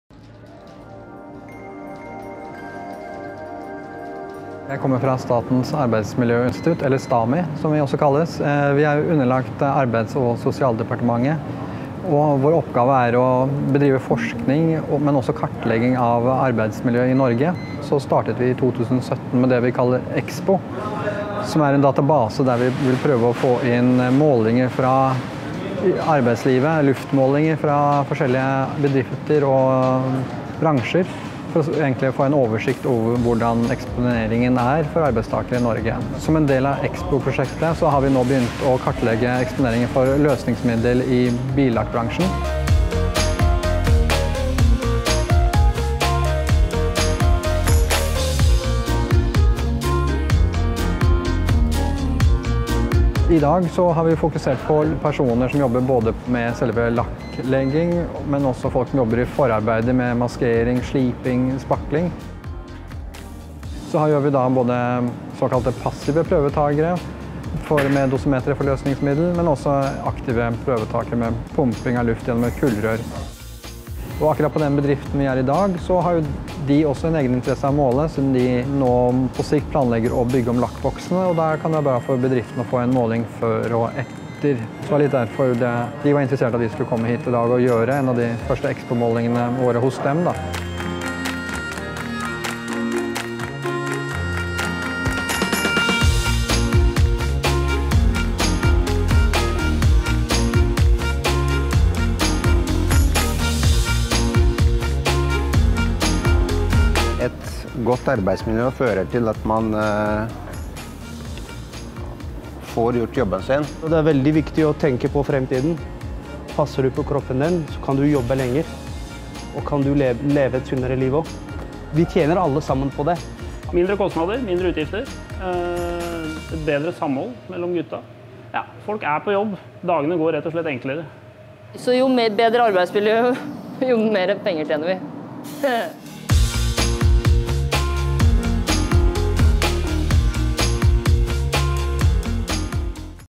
EXPO Online - Billakkering
STAMI har i samarbeid med Norges Bilbransjeforbund og Fellesforbundet startet en kartlegging med prøvetaking av arbeidsatmosfæren innenfor lakkering i bilbransjen. Kartleggingen vil gi kunnskap om dagens eksponeringsbilde innenfor billakkering og gir den enkelte bedrift bedre oversikt og kunnskap om eget arbeidsmiljø.